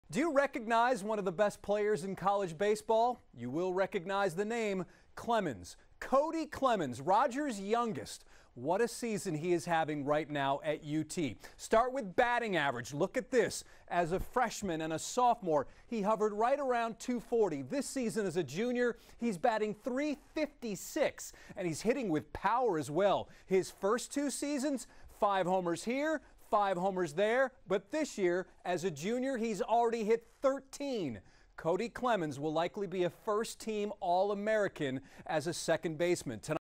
Clemens among nation's best
Texas second baseman Kody Clemens, former MLB pitcher Roger Clemens' youngest son, is one of the best college baseball players in the country.